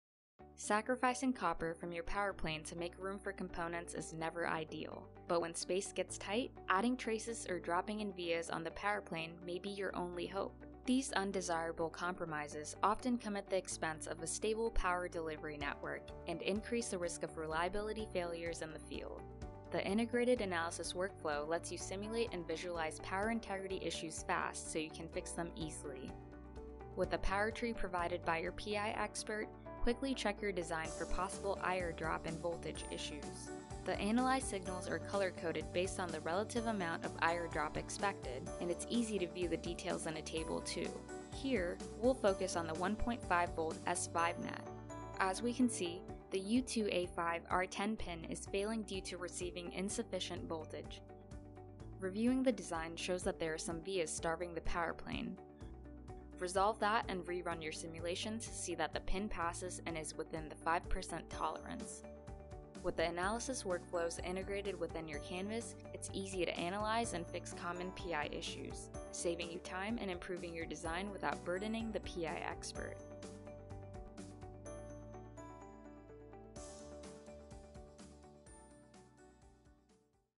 Simulate and Analyze IR Drop on Your PCB Design | Allegro PCB Designer
Sacrificing copper from your power plane to make room for components is never ideal. But when space gets tight, adding traces or dropping in vias on the power plane may be your only hope. These undesirable compromises in your PCB Design often come at the expense of a stable power delivery network and increase the risk of reliability failures in the field. Watch this video to learn how to simulate and analyze IR Drop on your PCB Design.